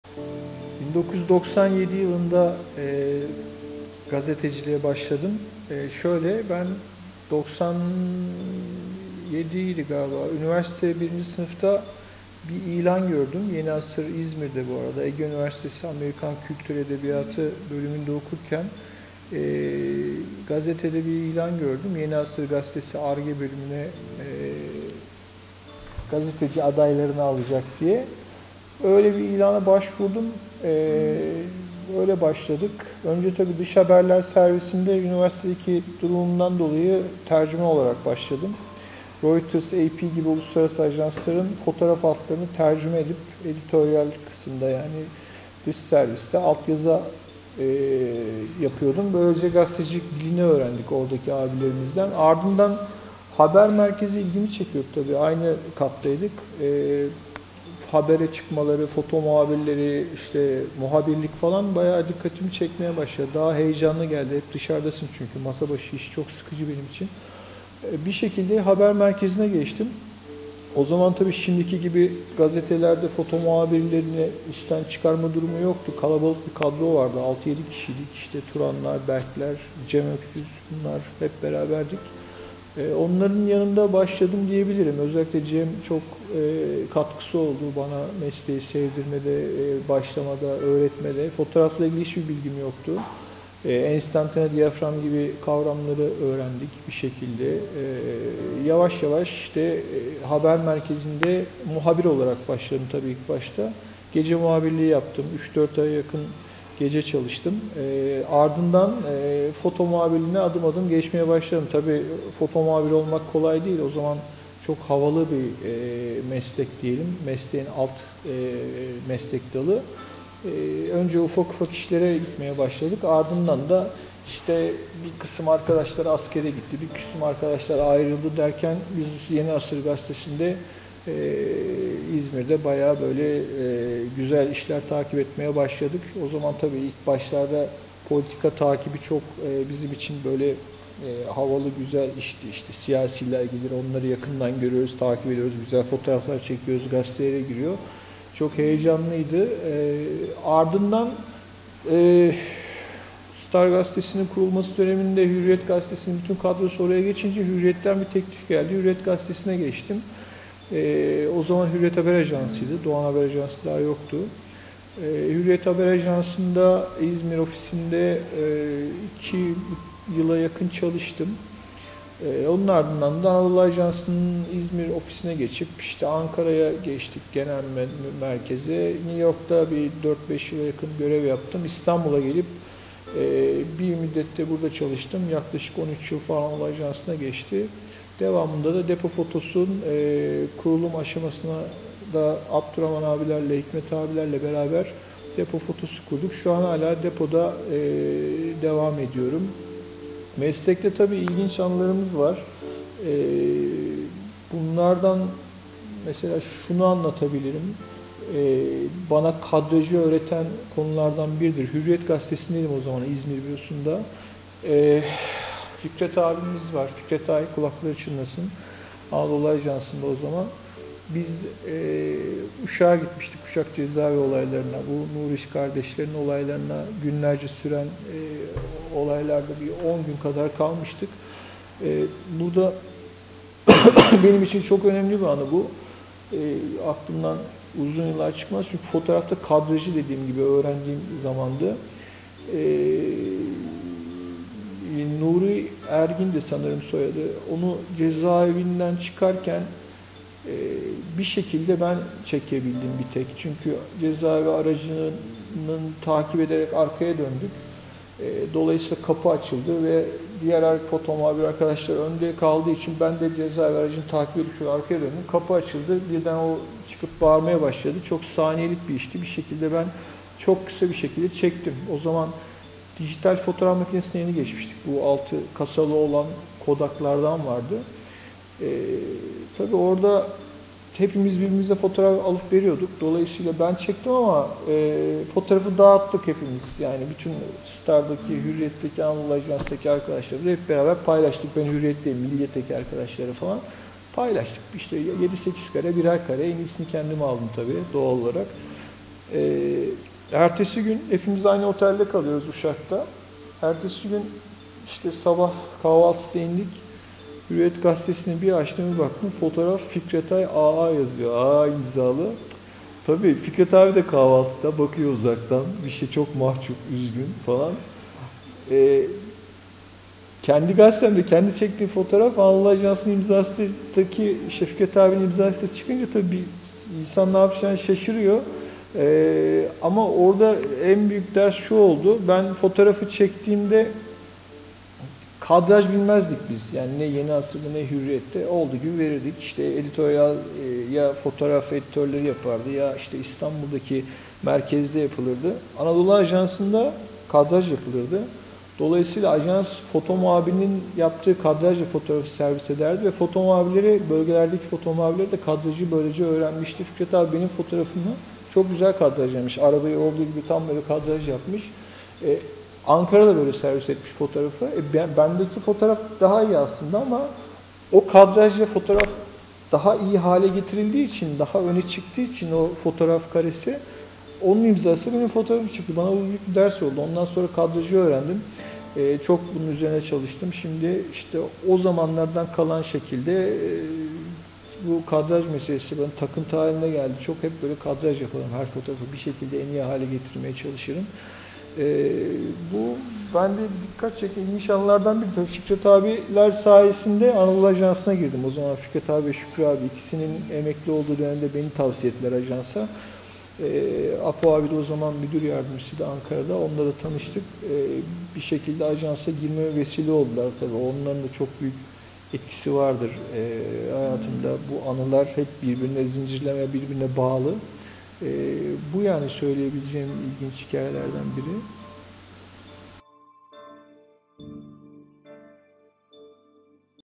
Tolga Adanalı / Foto Muhabiri / O Daha Güzel Kadraj Yapmıştı
Tolga Adanalı / Foto Muhabiri
Karadeniz Ereğli'de doğmuştur.
TED Koleji'ni bitirdikten sonra, Ege Üniversitesi Amerikan Kültürü ve Edebiyatı bölümünden mezun olmuştur. New York School of Visual Arts Üniversitesi'nde Master of Photography, Video and Related Media bölümünde yüksek lisans eğitimini tamamlamıştır.
1997 yılında İzmir'de üniversitede eğitimi sırasında gazeteciliğe adım atmıştır. Sırasıyla Yeni Asır, Hürriyet İzmir Bürosu (DHA) ve Anadolu Ajansı'nda foto muhabiri ve muhabir olarak çalışmıştır.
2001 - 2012 yılları arasında Anadolu Ajansı'nın İzmir, Ankara, İstanbul ve New York bürolarında görev yapan Adanalı, 2012'de fotoğraf ajansı Depo Photos'un kurucuları arasında yer almıştır. Depo Photos'ta yönetici ve foto muhabiri olarak çalışan Adanalı, bireysel olarak da fotoğraf çalışmalarına devam etmektedir.
Irak, Afganistan, Filistin, Libya, Somali gibi çatışma ve savaş bölgelerinde gazeteci olarak çalışmıştır. NATO, Birleşmiş Milletler, G20, gibi siyasi zirveler başta olmak üzere politika alanında uzun yıllar basın fotoğrafçılığı yapan Tolga Adanalı, Olimpiyatlar, NBA, Euroleague, Şampiyonalar Ligi ve diğer şampiyonalar ile çok sayıda spor organizasyonunda da fotoğraf çekmiştir.
Foto muhabirliği dışında mimari, moda ve reklam alanında da fotoğrafçılık yapan Tolga Adanalı, çoğu büyük organizasyonda resmi fotoğrafçı olarak yer almaktadır.
Türkiye Foto Muhabirleri Derneği'nde (TFMD) Başkan Yardımcısı olarak yönetim kurulunda görev alan Adanalı, ayrıca Türkiye Spor Yazarları Derneği (TSYD) ve National Press Photographers Association (NPPA) üyesidir. Ulusal ve uluslararası yarışmalarda da çok sayıda ödülü bulunmaktadır.